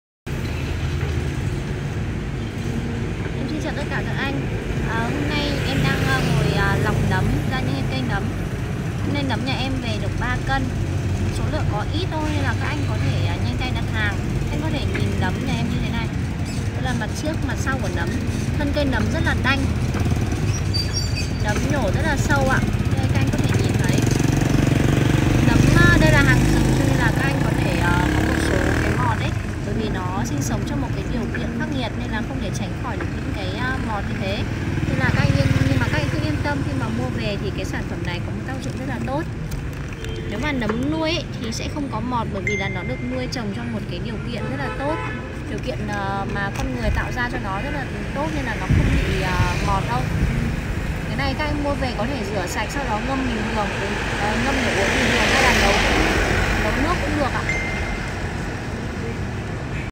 CÁCH SỬ DỤNG NẤM LIM XANH ĐÚNG CÁCH - THẢO DƯỢC BẢO KHÁNH
Nấm lim xanh rừng là bài thuốc quý có nhiều công dụng thần kì giúp cải thiện hệ miễn dịch có tác dụng trong điều trị ung thư. Đặc biệt nấm lim xanh còn giúp giải độc gan giúp gan khỏe hơn

Cửa hàng Thảo dược Bảo Khánh nơi cung cấp Nấm lim xanh rừng chuẩn uy tín đảm bảo chất lượng cho khách hàng